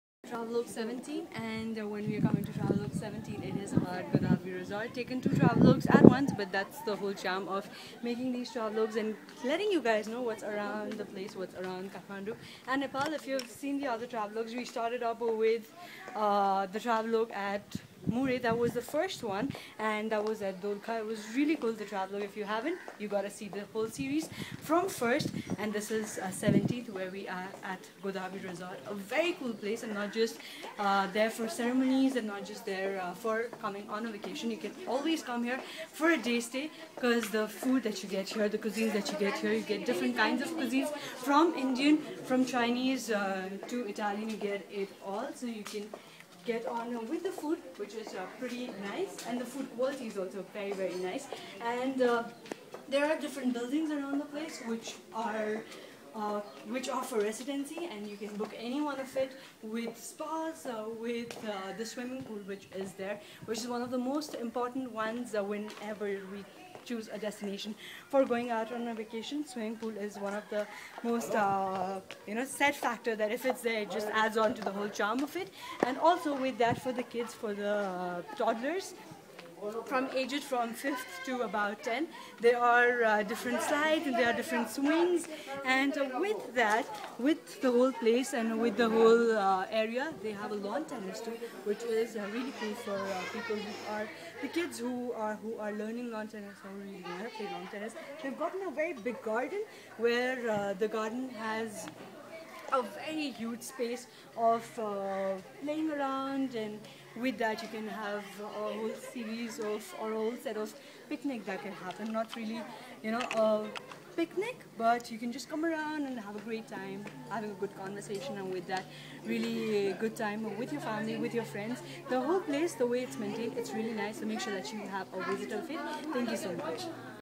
Travel with Shikha XVII :- Godavari Resort
Godavari is located approximately ten kilometers from both Patan and Kathmandu, and many outdoor enthusiasts here to enjoy the breathtaking views and beautiful landscapes.It attracts many local residents and visitors over the weekends.